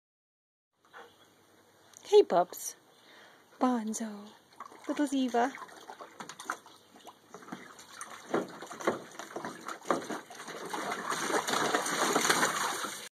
Bonzo and Ziva in the pool
Two of Pema's pups cooling off in the pool.